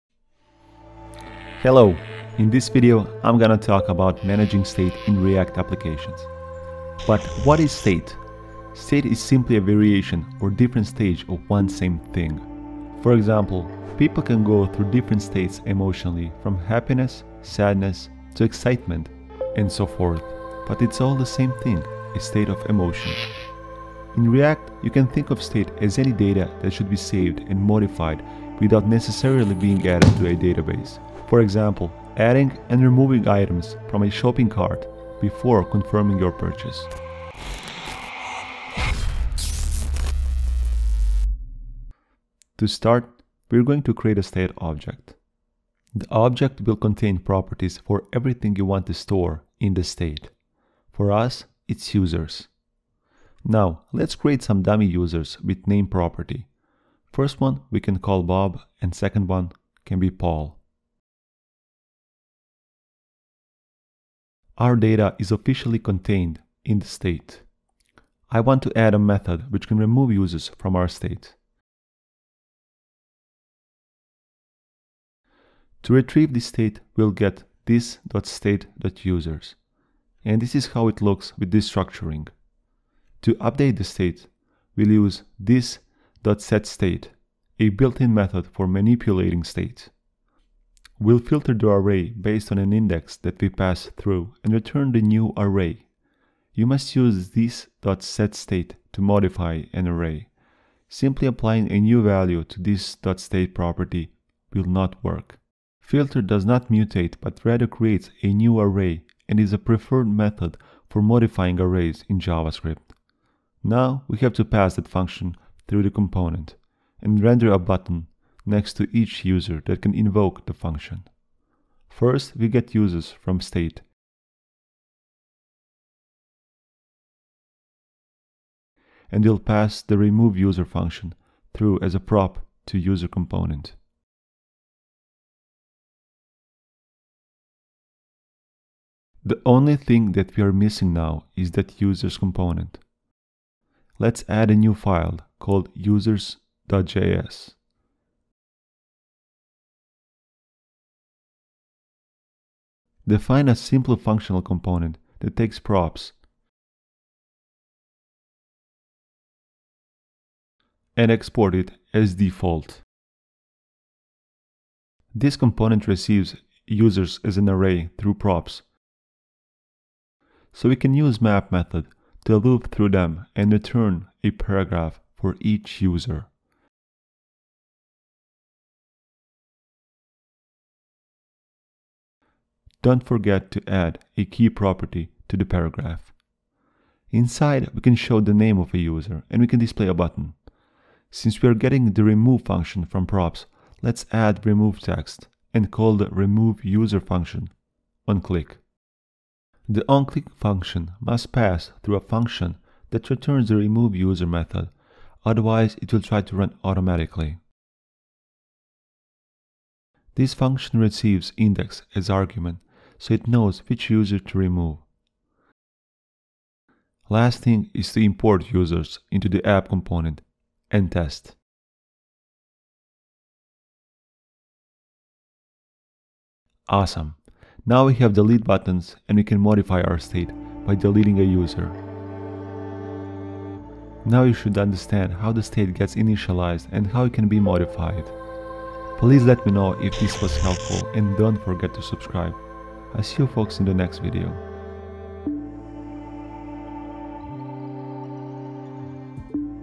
Managing state in React #5 React Course | Code with Sloba
Hello in this video I’m gonna talk about managing state in React applications. How can you handle state in your Class component in React? Or how can you manage state in a functional component?

Managing state in react is pretty straightforward.

🧿 I discussed:

✔️  What is the state in React?
✔️  How to create a state object
✔️  How to use setState
✔️  How to update states in CLASS component

0:00 Intro
0:08 What is state
0:40 Create state in Class component
2:30 Coding Users functional component
3:45 Testing the app